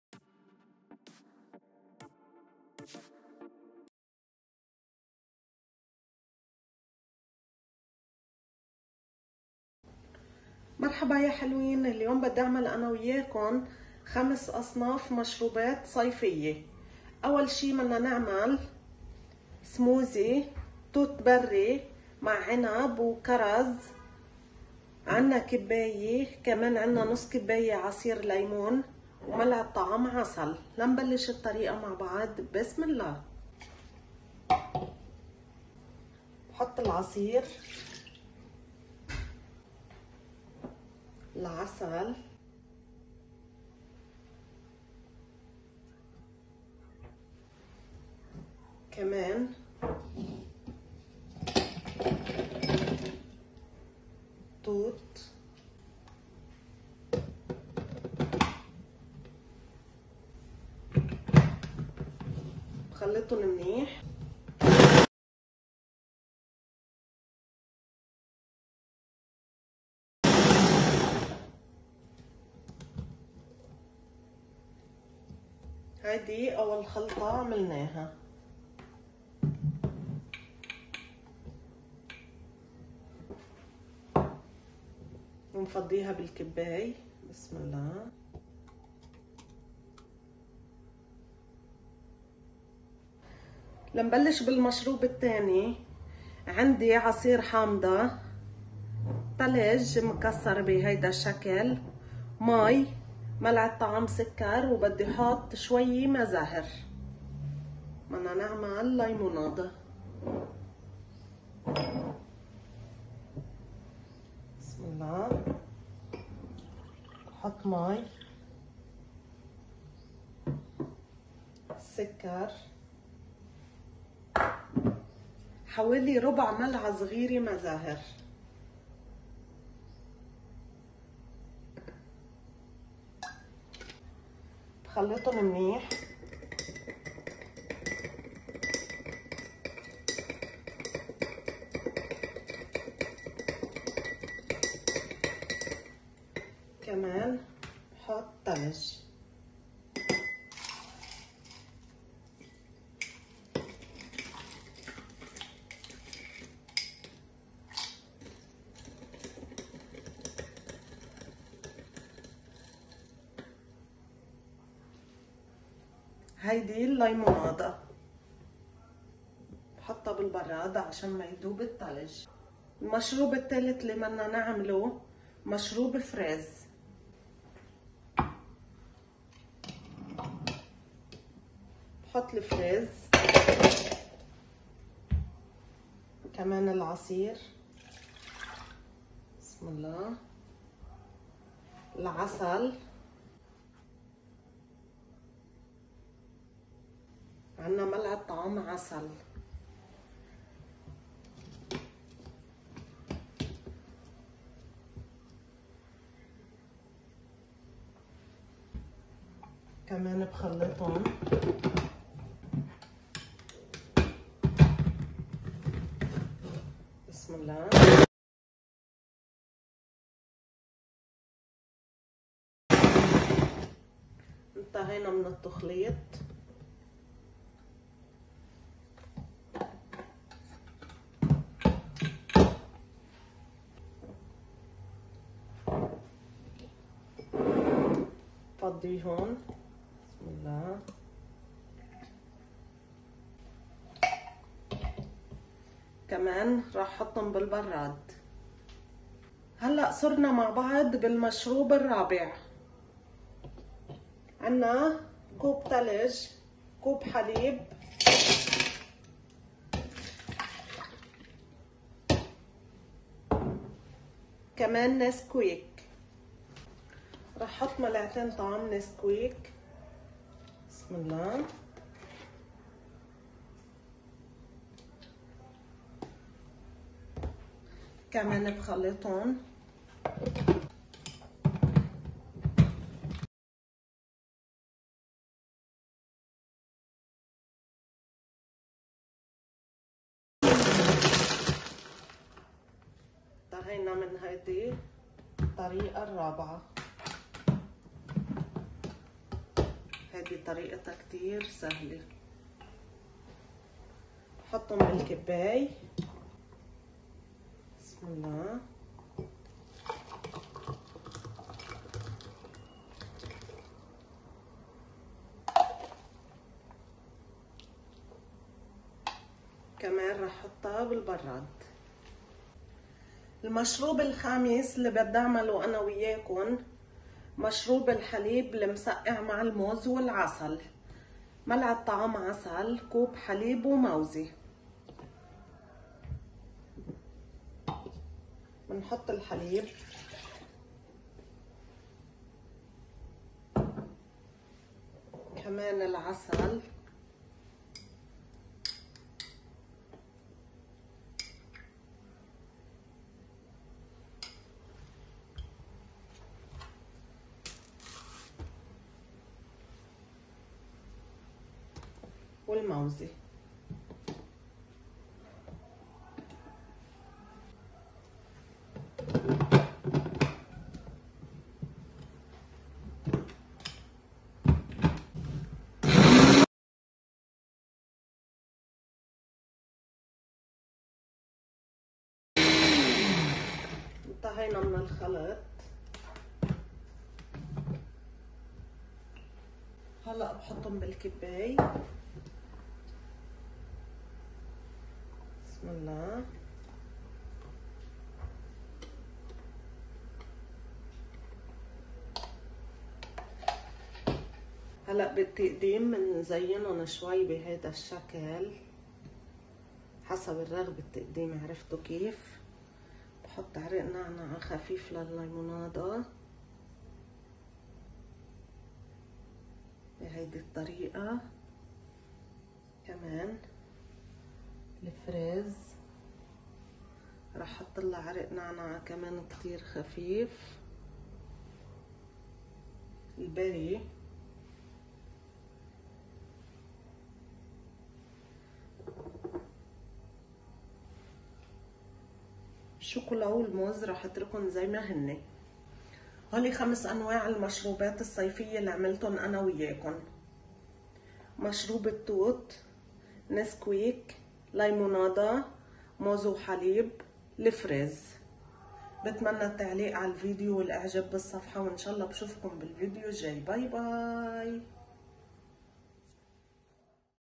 5 Summer Smoothies - ٥ مشروبات باردة ومنعشة وبنكهات مختلفة للصيف
5 Summer Smoothies - ٥ مشروبات باردة ومنعشة وبنكهات مختلفة للصيف
المقادير
١ كوب توت وعنب وكرز
١/٢ كوب عصير ليمون
١ ملعقة طعام عسل
١ عصير حامضة
١ كوب ماي
١ ملعقة طعام سكر
١/٤ ملعقة صغيرة ماء زهر
ثلج
١ كوب فريز
١ ملعقة طعام عسل
١/٢ كوب عصير ليمون
٢ ملعقة نسكويك
١ كوب حليب
ثلج
١ كوب حليب
موزة
١ ملعقة طعام عسل
بتمنى التعليق على الفيديو والإشتراك بالصفحة
Ingredients
berry, cherry, and grape 1 cup
1/2 cup orange juice
1 tablespoon honey
juice of 1 lemon
1 cup of water
1 tablespoon of sugar
1/4 teaspoon of blossom water
ice
1 cup of strawberry
1/2 cup of orange juice
1 tablespoon of honey
2 tablespoon of Nesquik
1 cup of milk
ice
1 cup of milk
banana
1 tablespoon of honey